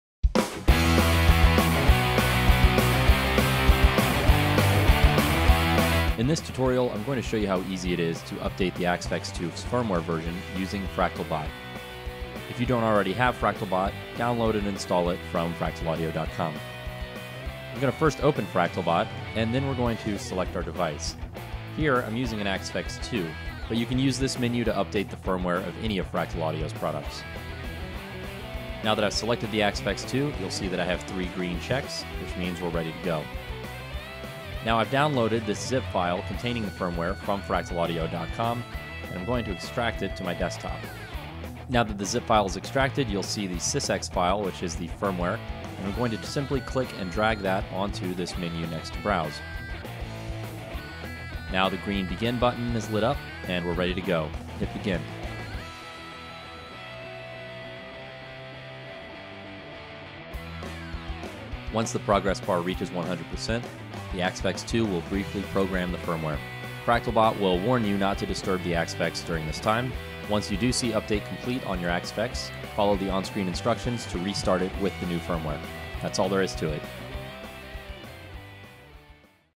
Updating the Axe-Fx II with FractalBot
It's easy to update the Axe-Fx II with Fractal Audio's FractalBot utility.